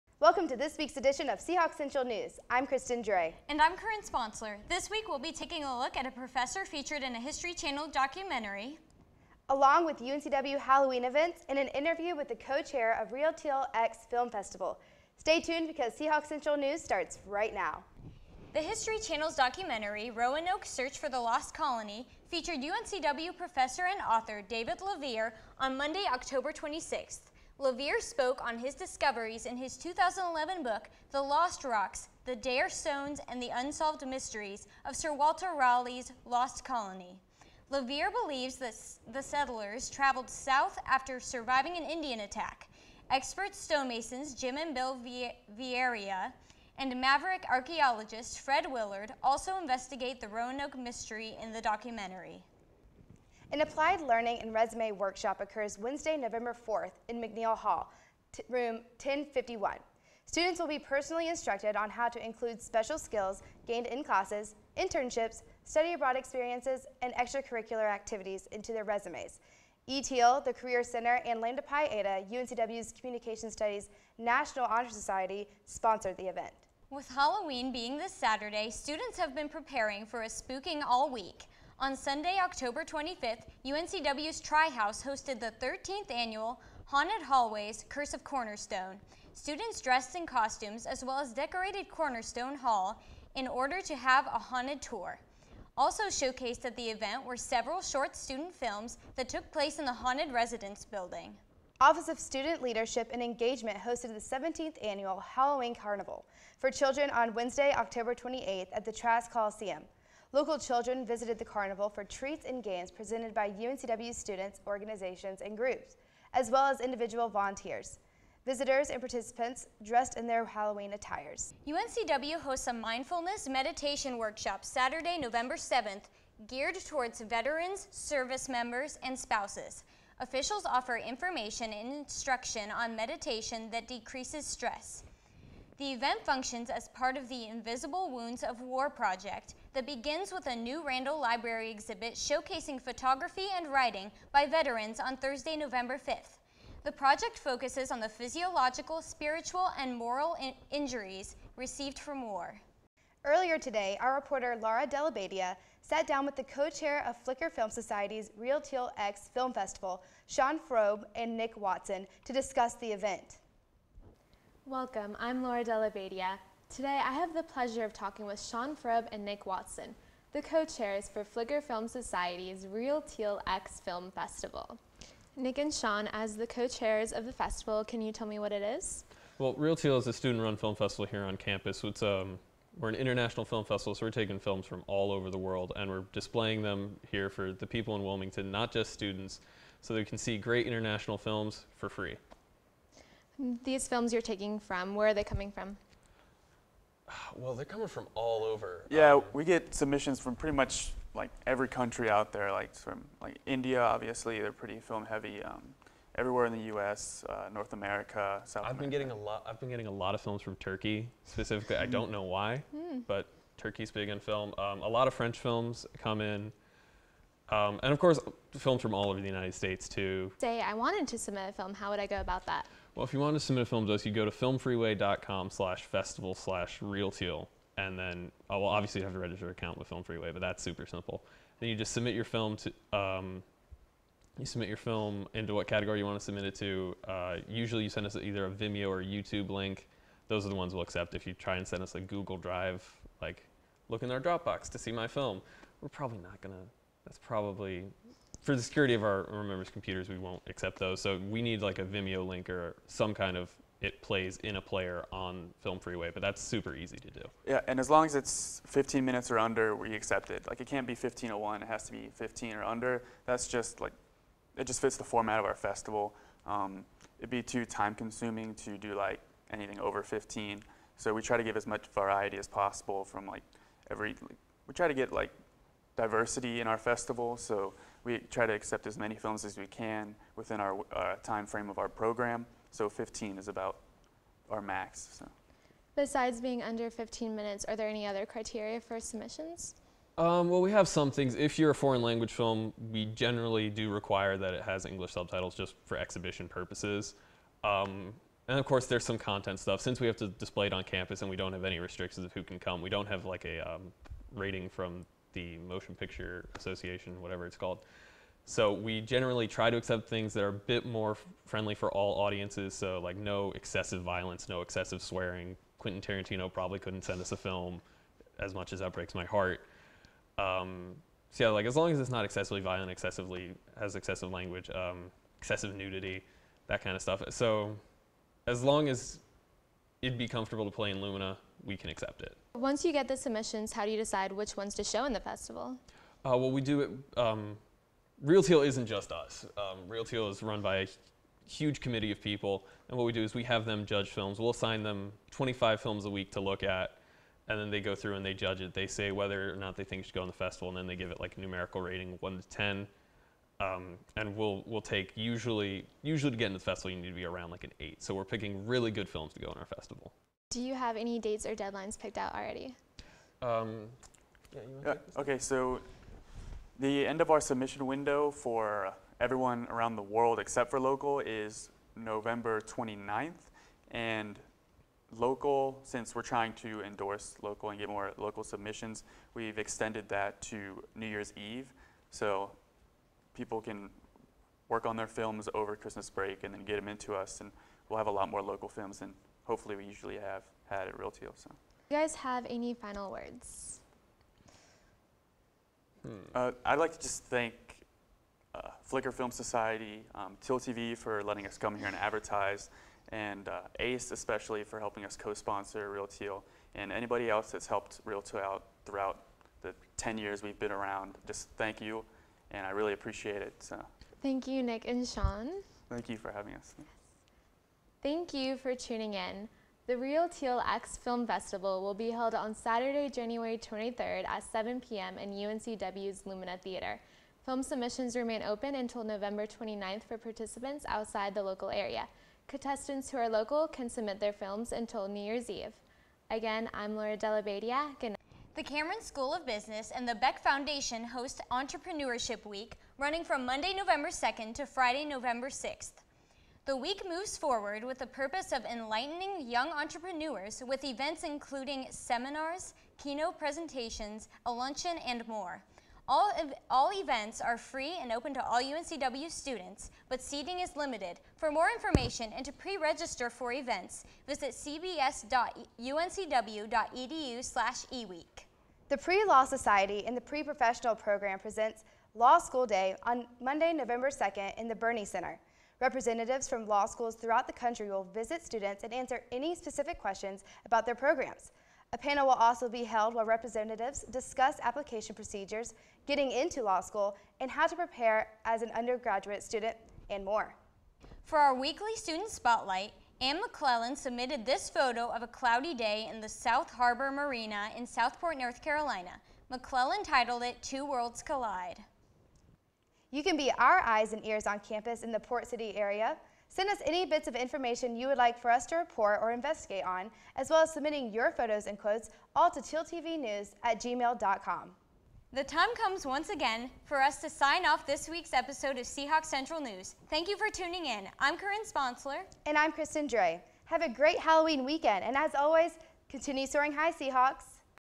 Seahawk Central News: Hauntings, Meditation, and Reel Teal
Join anchors Kristen Dray and Corin Sponsler as they keep you up to date on news at UNCW and in Wilmington.

Reporter Laura DellaBadia sits down with Reel Teel co-Chairs Sean Froeb and Nick Watson.